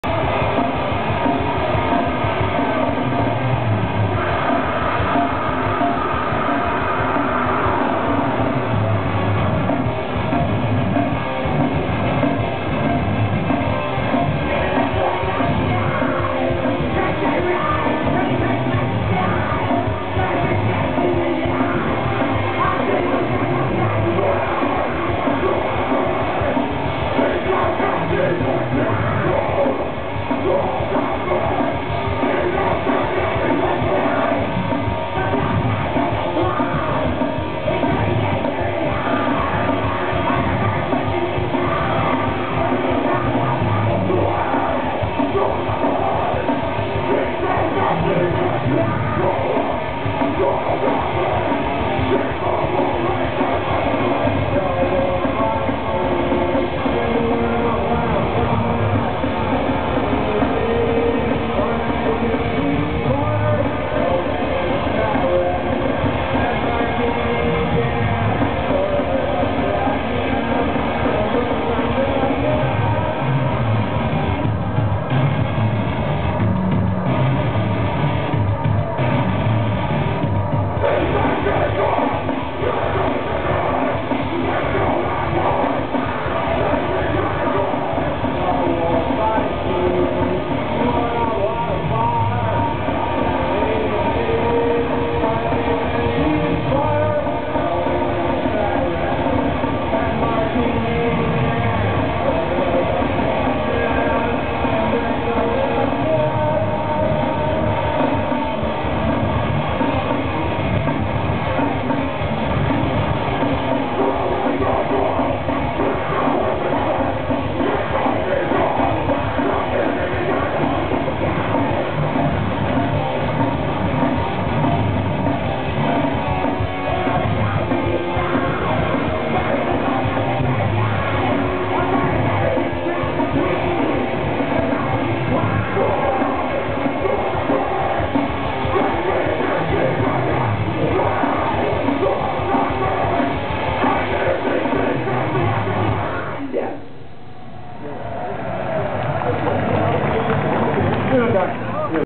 Napalm Death - Live @ Wacken Open Air 2012
Live video filmed at the Wacken Open Air festival 2012 in Germany